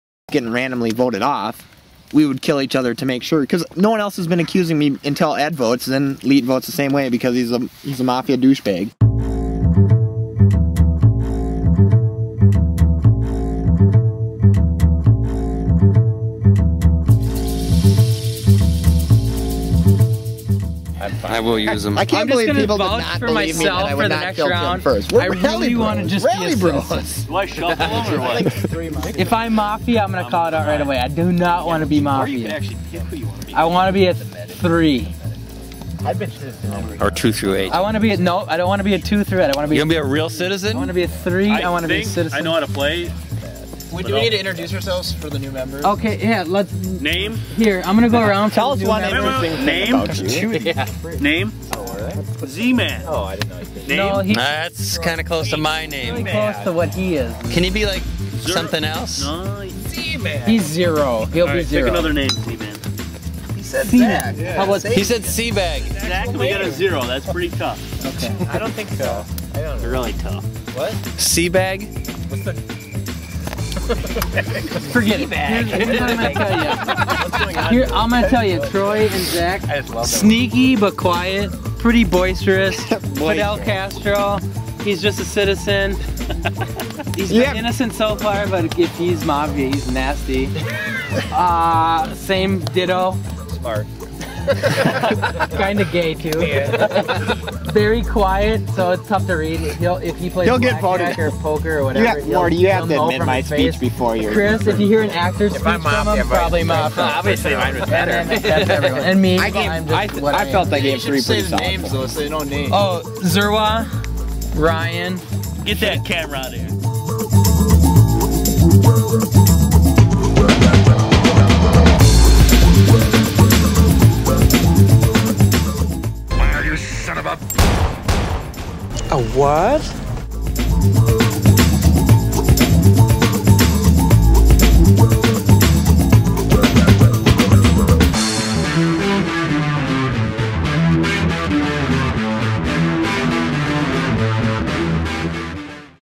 MaFiA 9.13.08
The guys get together to play a little Mafia around the fire one September night.